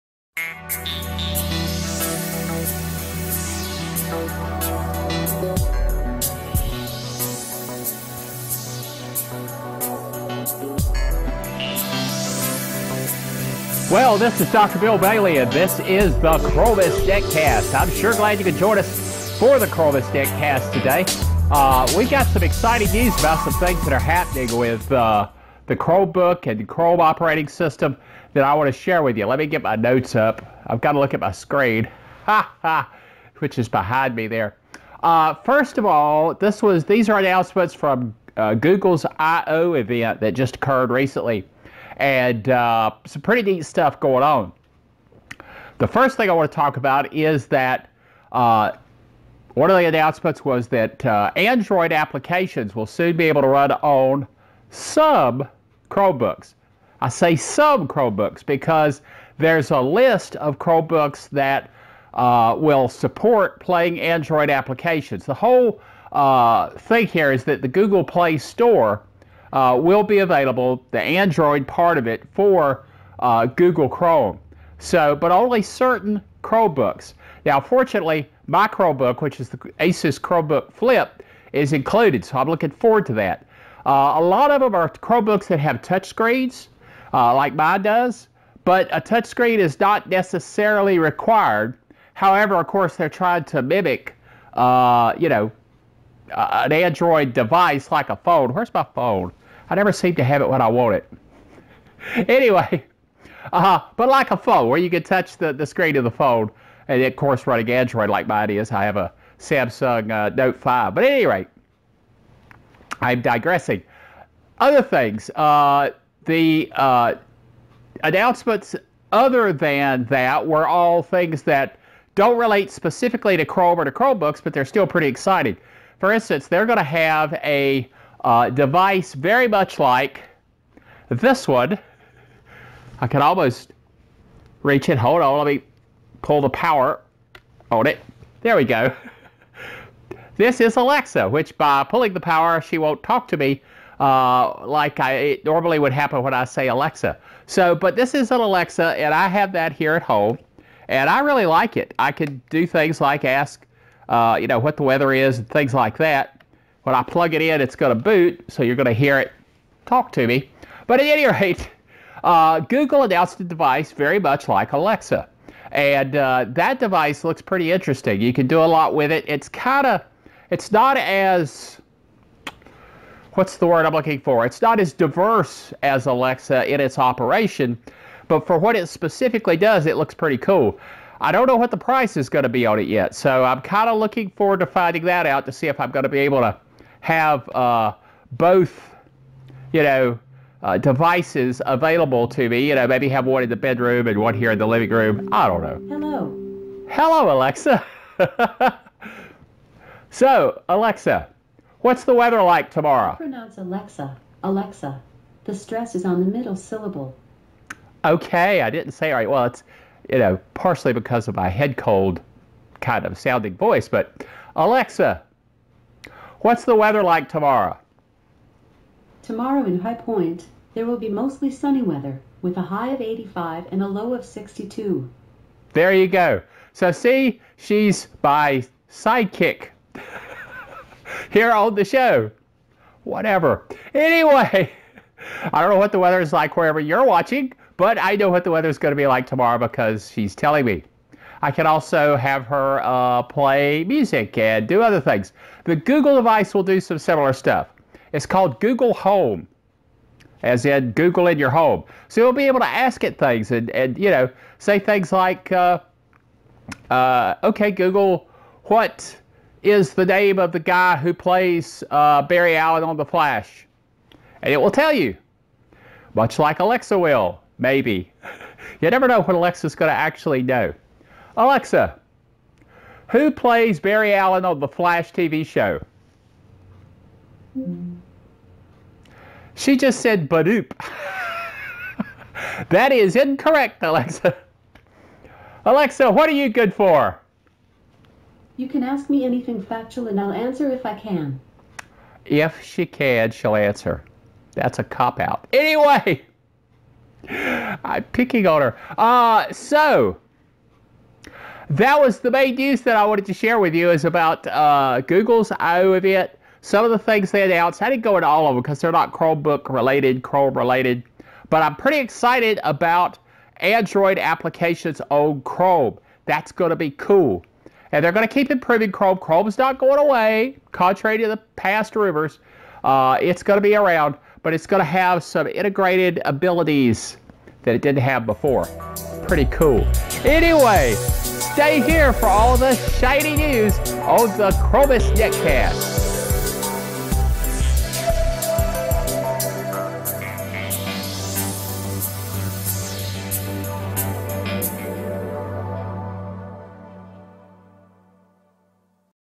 The Chromest Netcast #10 - "News from Google I/O for Chrome"
Google Play coming to Chromebooks and will be able to run Android apps but only on specific devices... and the Google Home vs. Alexa!